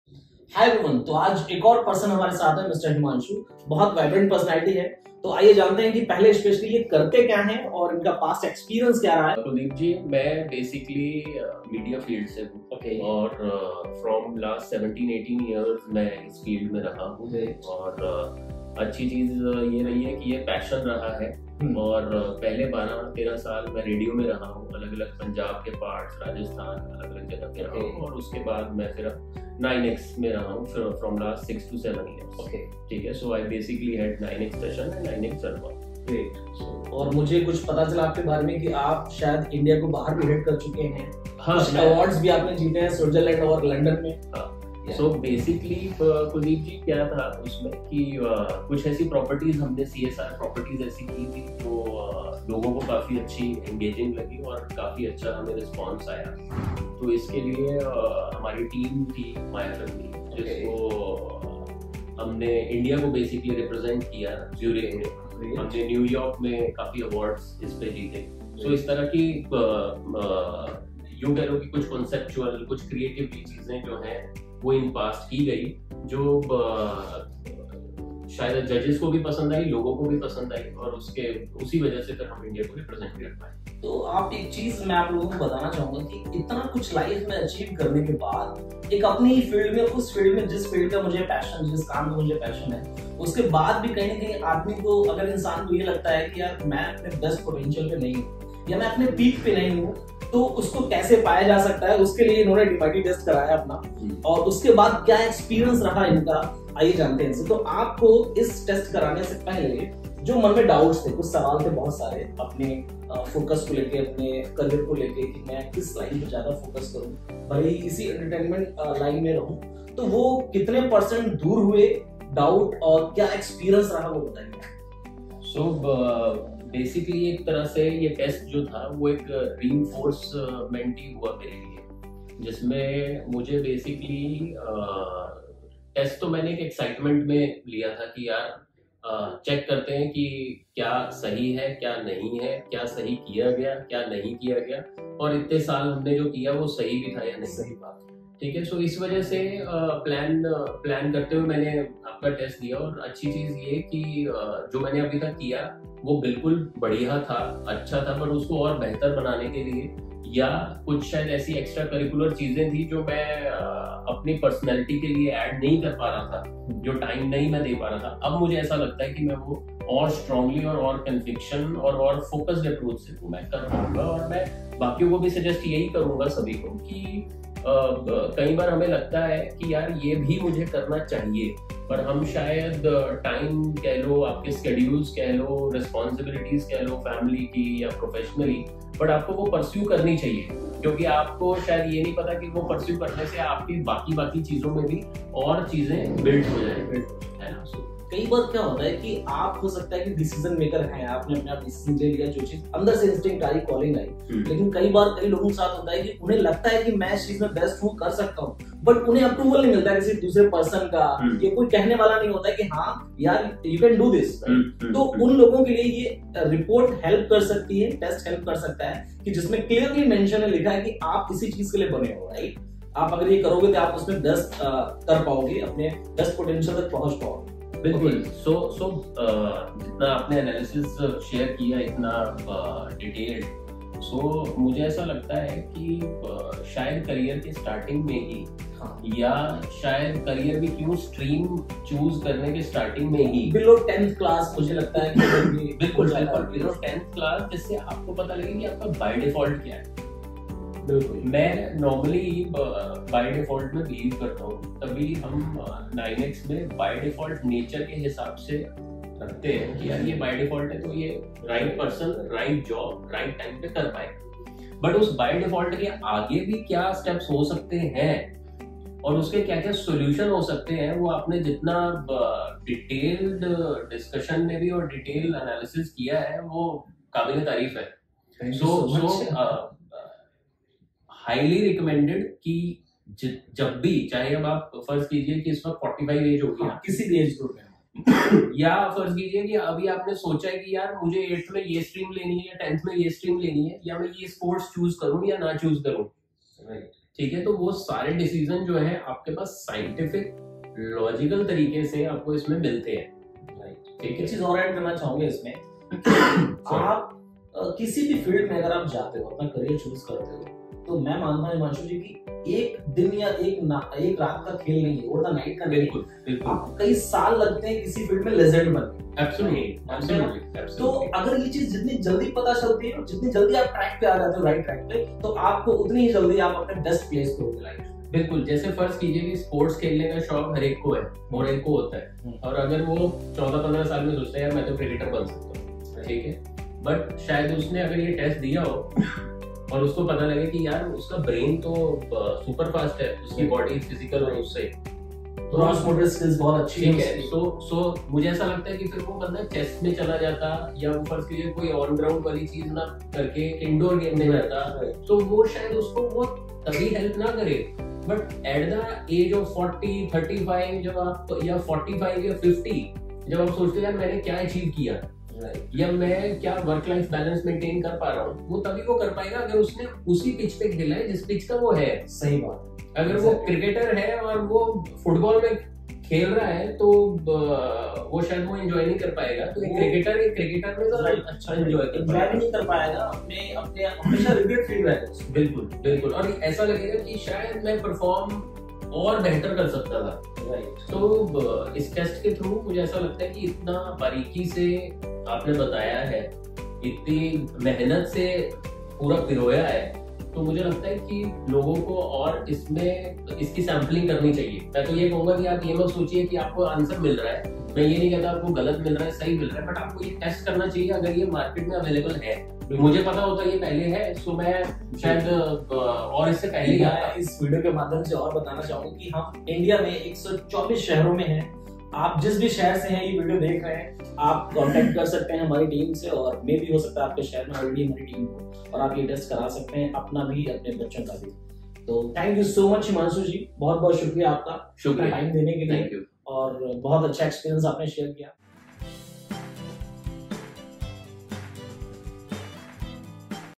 DMIT Test Feedback | DMIT Test Benefits | What is DMIT Test in Hindi
A Big Thank You to Mr. Himanshu Gupta for this feedback video.

I want to express my heartfelt gratitude to the amazing Mr. Himanshu for taking the DMIT test! 🌟🧠

Himanshu, as a vibrant personality in the media and music industry, you've always impressed us with your talent and energy. Now, by exploring your brain's capabilities, you've taken it a step further. How inspiring! 🎶💡

Once again, thank you, for your enthusiasm and willingness to explore the limitless possibilities of the human mind. We are privileged to witness your incredible journey and can't wait to see the extraordinary heights you'll reach!